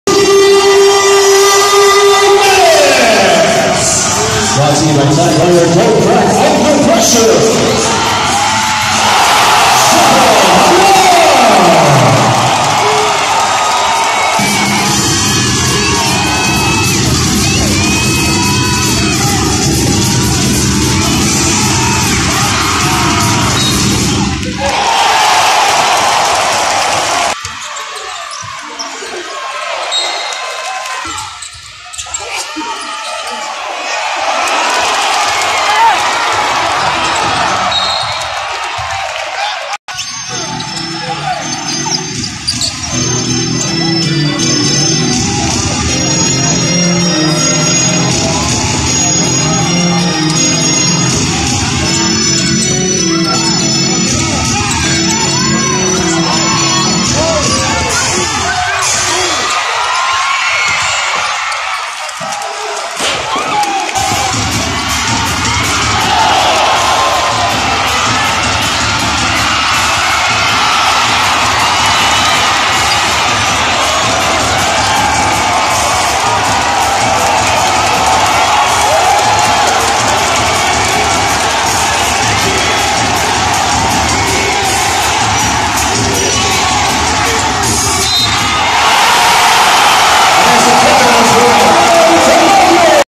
Japeth Aguilar's putback slam, LA Tenorio's dagger 3-point play in Game 3 | 2018 PBA PH Cup semis
Aguilar's dunk and Tenorio's and-one play that sealed Ginebra's 95-87 win over San Miguel Beer to trim the series deficit at 1-2.